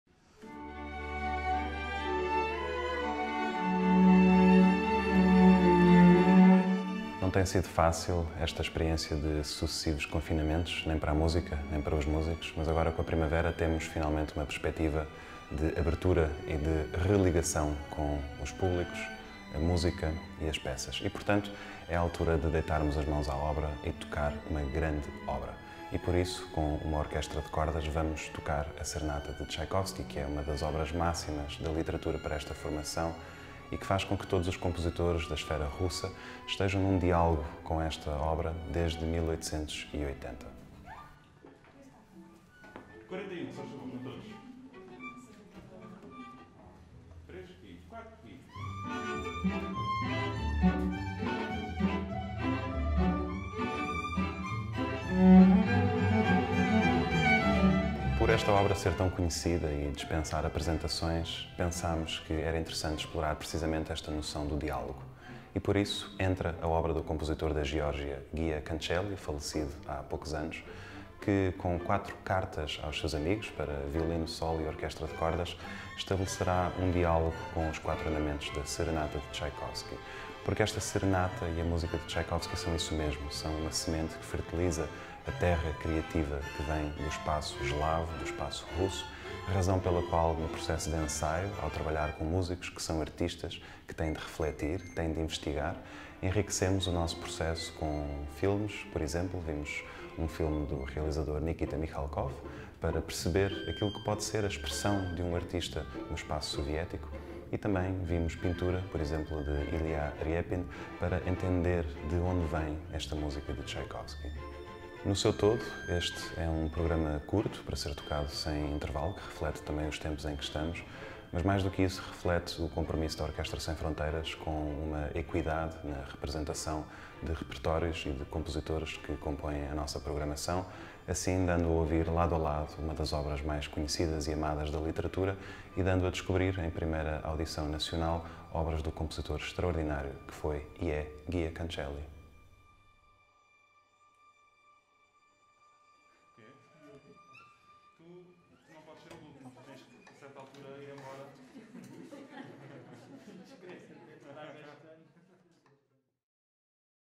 Apresentação dos Concertos de Primavera
Martim Sousa Tavares apresenta os Concertos de Primavera, que marcam o regresso da Orquestra Sem Fronteiras às actividades presenciais, combinando a música de Tchaikovsky com a estreia nacional de quatro obras de Giya Kancheli, com o solista André Gaio Pereira.

Videografia de Adriana Romero.

osf.pt